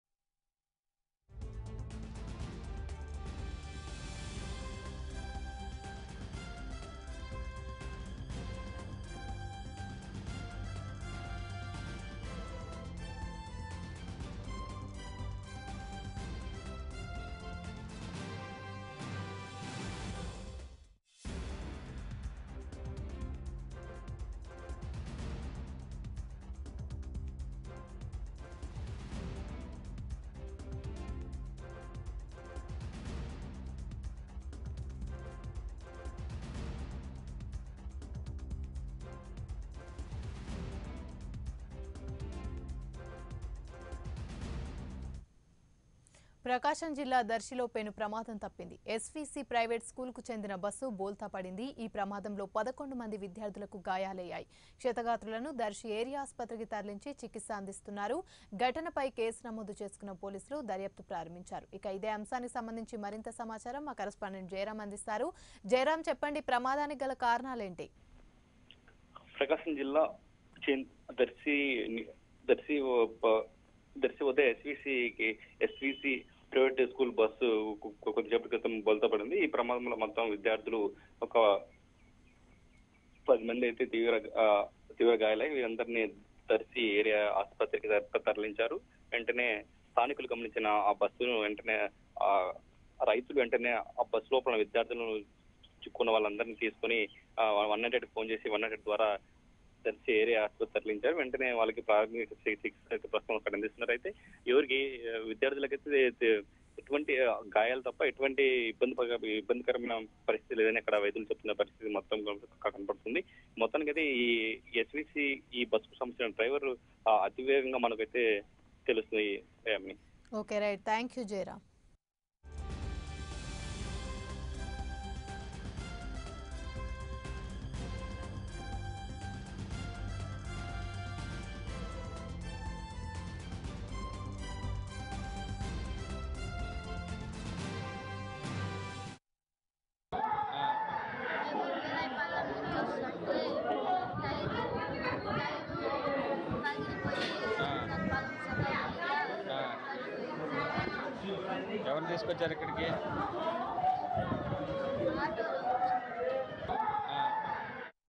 SVC School bus rolls over at Darshi in Prakasham, 11 students injured | Prime9 News
Eleven children have been treated for minor injuries after a school bus rolled over in Prakasham.